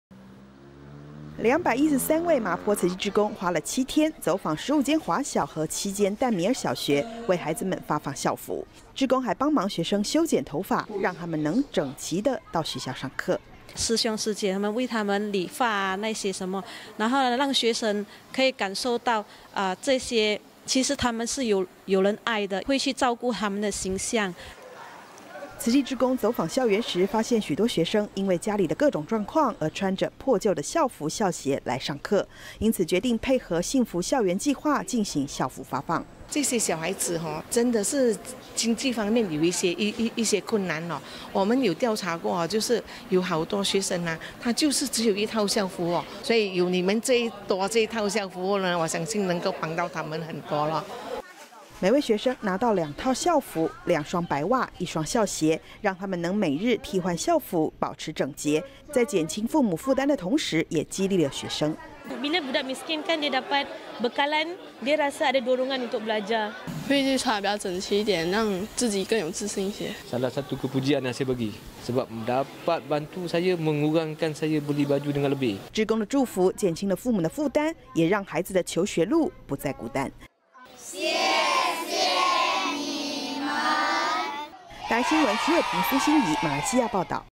20170707【教育】一身制服乾淨整齊 開創亮麗未來
在馬來西亞麻坡，很多學生因家境清寒，沒錢購置新校服，即使衣服已經舊了，鞋子已經破了，仍穿著到學校。配合幸福校園計劃推動，慈濟志工在15間華小及7間淡米爾小學，發放兩套校服、兩雙白襪及一雙校鞋，給314位有需要的學生，增加孩子們學習的信心。

213位麻坡慈濟志工花了7天，走訪15間華小及7間淡米爾小學，為孩子們發放校服。志工還幫忙學生修剪頭髮，讓他們能整齊地到學校上課

育英小學老師 郭愛君：「師兄師姊為他們理髮，然後讓學生可以感受到這些，其實他們是有人愛的，會去照顧他們的形象。」

慈濟志工走訪校園時，發現許多學生因為家裡的各種狀況，而穿著破舊的校服校鞋來上課，因此決定配合幸福校園計劃進行校服發放

南華小學校長 吳木嬌：「這些小孩子 真的是經濟方面，有一些困難 我們有調查過，就是有好多學生 他就是，只有一套校服 所以有你們，多(發)這一套校服，我相信能夠幫到他們很多。」

每位學生拿到兩套校服，兩雙白襪，一雙校鞋，讓他們能每日替換校服，保持整潔，在減輕父母負擔的同時，也激勵了學生。

東甲園丘淡米爾小學老師 菩妮達：「當清寒的學生得到幫助時，會讓他們覺得要更積極的學習。」

育英小學學生 謝凱欣：「會穿得比較整齊一點，讓自己更有自信一些。」

家長 莫哈默：「志工的幫忙讓我減輕了負擔，我不必再幫孩子多準備一套校服。」

志工的祝福，減輕了父母的負擔，也讓孩子的求學路不再孤單。

徐瑋婷 蘇心怡 馬來西亞報導(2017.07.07)